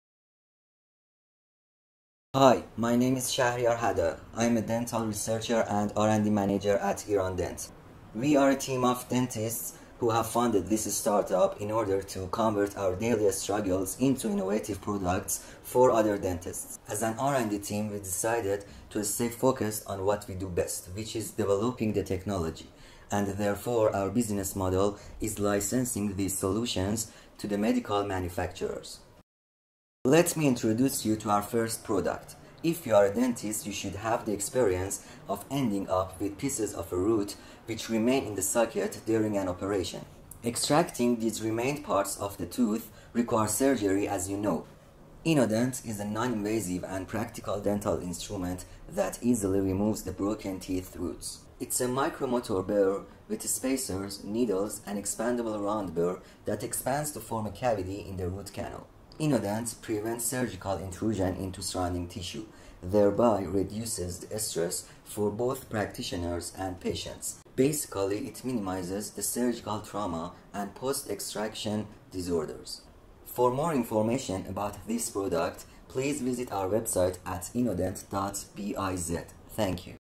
Get to know Innodent.
Hi everyone

The Innodent team was formed in 2018 as a group of dental experts to develop dental technologies in Iran gradually entered the global market.
The mission of our company is to explore the recent demands of the market industry and propose proper solutions by designing novel instruments that can assist dental practitioners all over the globe to enrich their experience and optimize their work. So far, our team has developed various innovatory dental instruments based on thorough research and precise clinical trials, along with several patents and awards.

We have designed a dental motor bur (EMB) for extracting a retained tooth root without surgery and its complications. Our device was succeeded to receive multiple awards and recognition, and we have published our related research in prestigious medical journals.

Moreover, we are currently working on a root canal detector (RCD) that provides a cost-effective solution for identifying the root canal curvature and facilitating the treatment course. We believe that our devotion and expertise put us on a road to success in our country and worldwide.